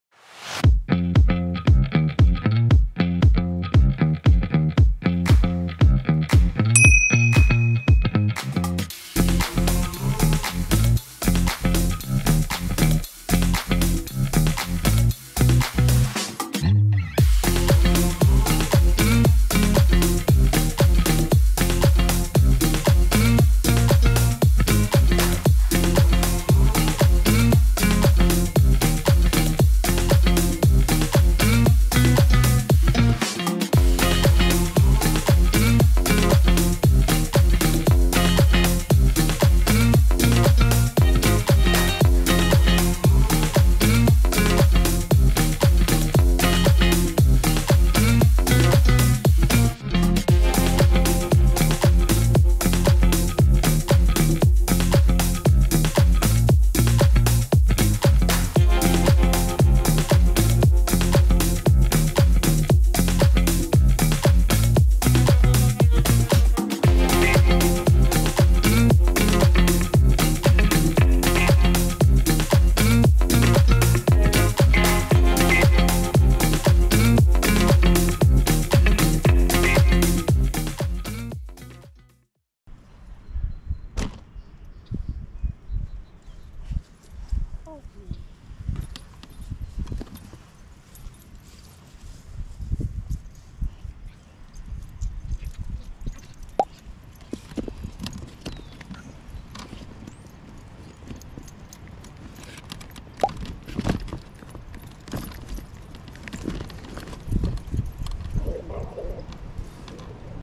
Casper Cab
Some quick clips of a little raptor puppy living his best life.